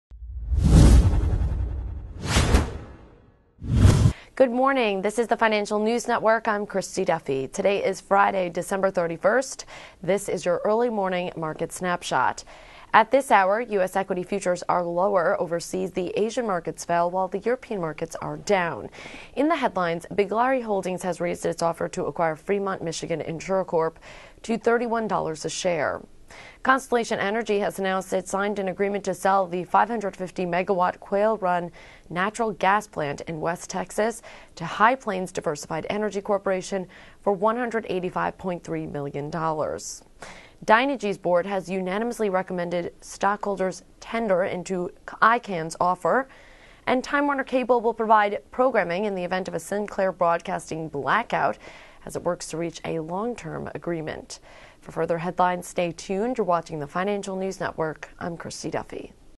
Morning Market Snapshot: December 31th, 2010
Good Morning. It's Friday, December 31, 2010. At this hour, U.S. equity futures are lower. Overseas, the Asian markets fell, while the European markets are down. Biglari Holdings (BH) raises offer to acquire Fremont Michigan InsuraCorp to $31 a share...Constellation Energy (CEG) announced it signed an agreement to sell the 550-megawatt Quail Run natural gas plant in West Texas to High Plains Diversified Energy Corp. for $185.3M...Dynegy's board (DYN) unanimously recommended stockholders tender into Icahn's offer...Time Warner Cable (TWC) will provide programming in event of a Sinclair Broadcasting (SBGI) blackout as it works to reach a long-term agreement...Alliant Energy (LNT) receives district court cash balance pension plan order.